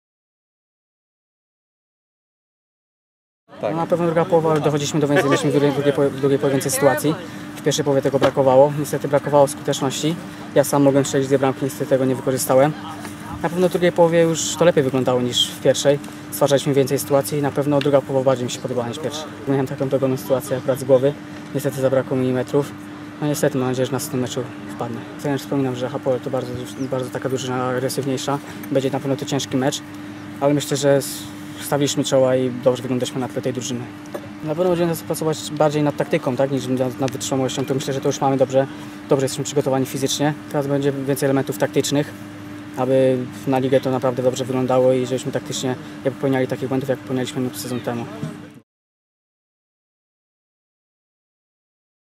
Wypowiedź Mariusza Rybickiego po meczu z Hapoelem Beer Sheva
Zdjęcia i montaż: Arkadiusz Błaszczyk